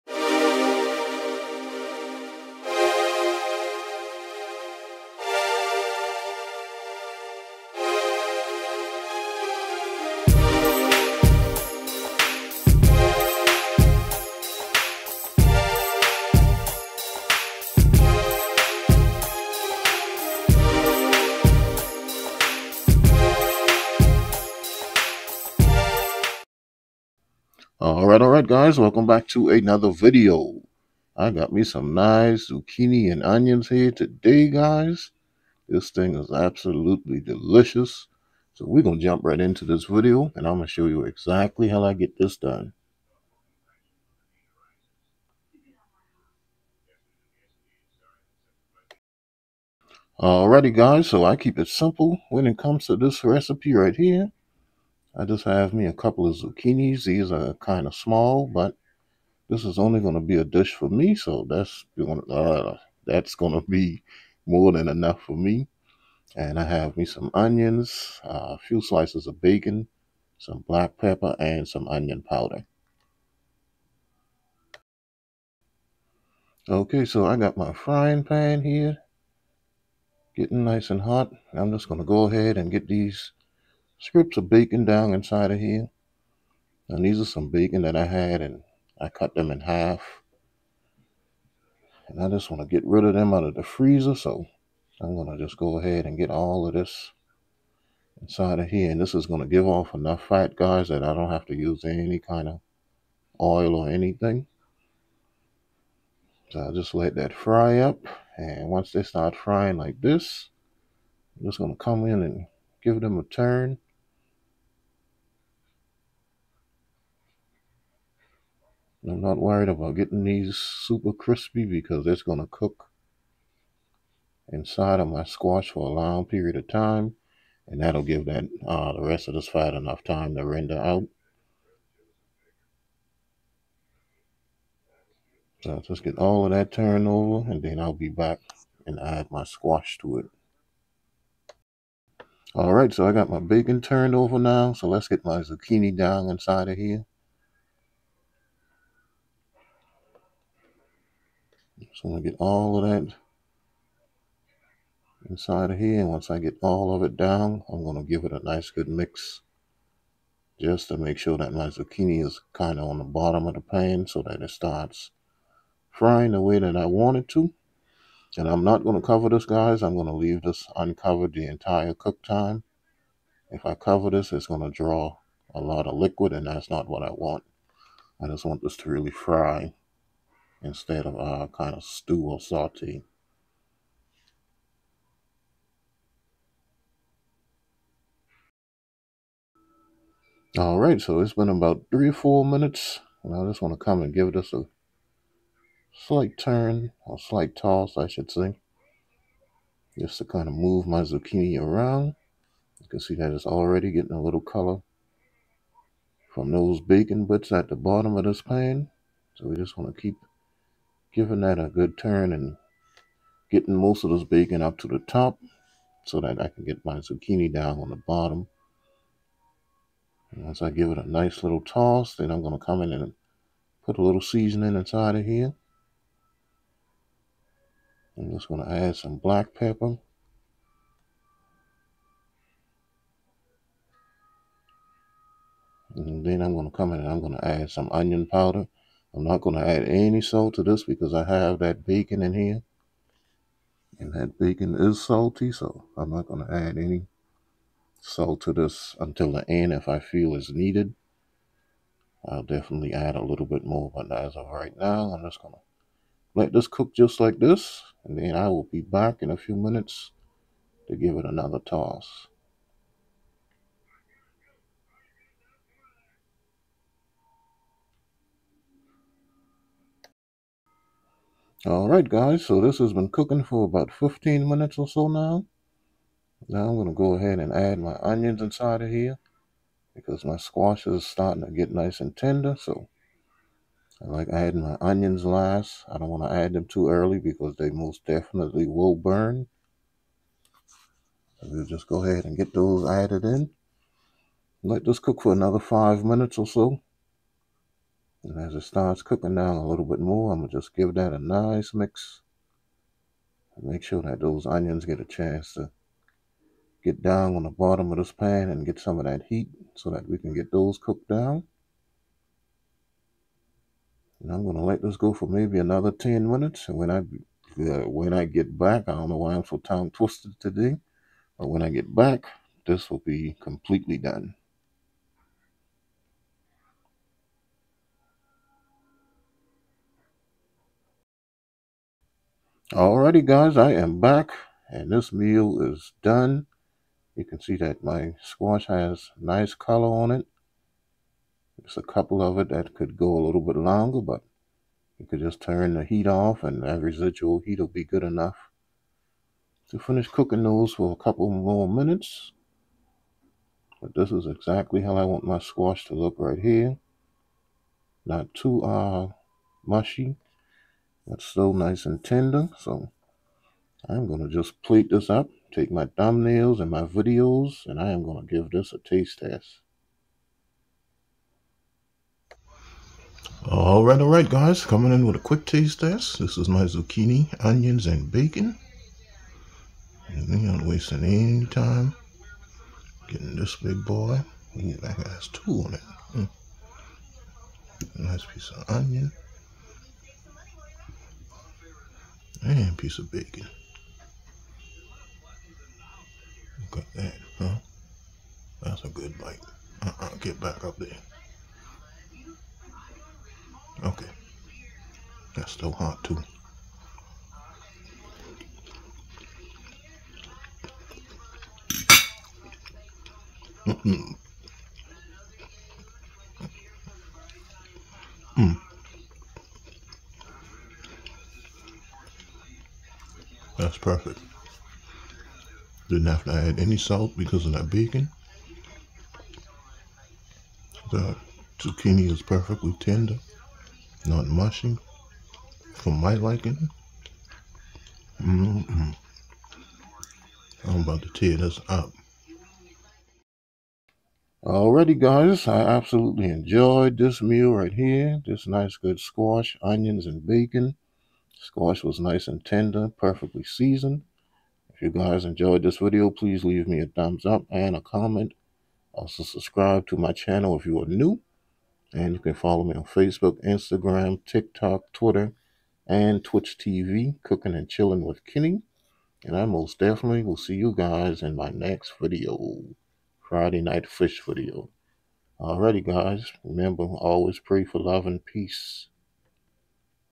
Zucchini with Bacon and Onions
Ingredients;
Zucchini
Bacon
1 Medium Onion
Seasonings of Choice

Directions
1. Fry bacon slightly on both sides to render some fat.
2. Add zucchini and give a mix.  Let cook for 3 - 5 minutes uncovered.
3. Add seasonings and mix well to distribute seasonings. Allow to cook an additional 10 - 12 uncovered until  zucchini begin to soften.
4. Add in onions and mix. Cook for 10 more minutes uncovered. Zucchini and onions should be perfectly tender. Adjust seasonings if need be,
5. Once done, serve hot as the main dish or as a side. Enjoy!

My Kitchen Gadgets

Cookin' And Chillin' With Kenny
McClellanville, SC 29458